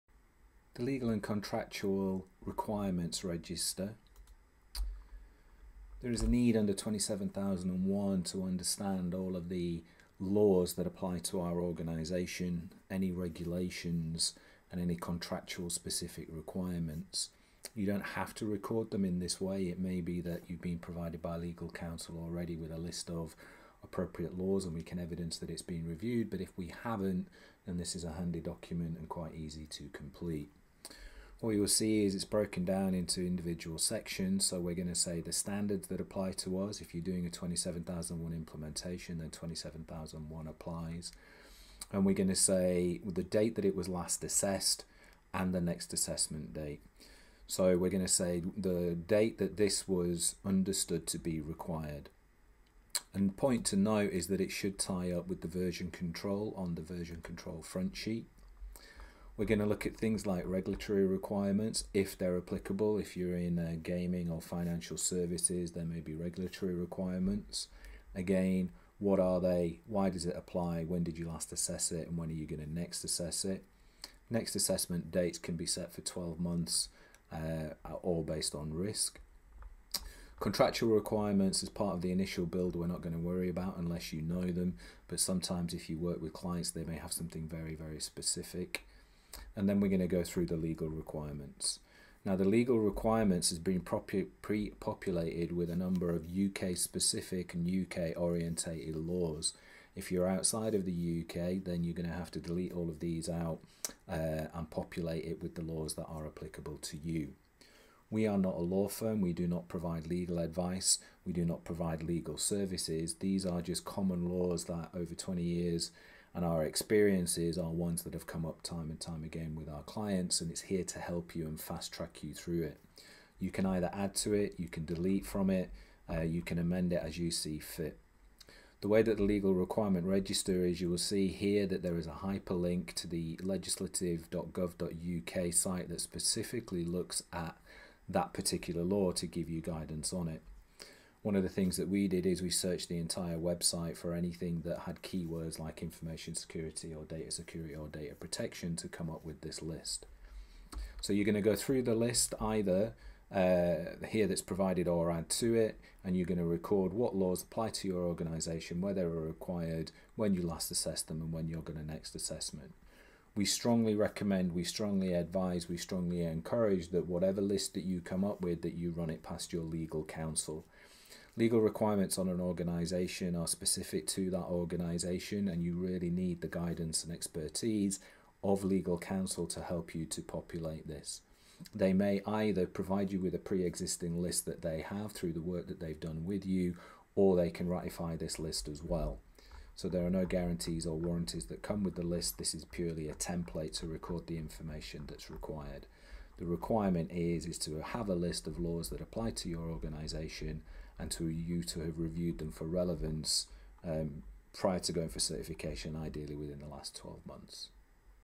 ISO 27001 Legal Register Template | How To and Walkthrough
ISO 27001 Legal and Contractual Register Template includes a walkthrough of the template and a guide on how to complete it.

ISO 27001 requires to understand what laws apply to our organisation. To go for ISO 27001 certification you may be required to demonstrate how you comply with laws that apply to you.

The ISO 27001 legal register is a simple document to record the laws that apply. This version has been pre populated with UK data laws and information security laws. Each law is hyperlinked back to the corresponding government website for ease of reference.

Learn what a legal register contains and how to complete it.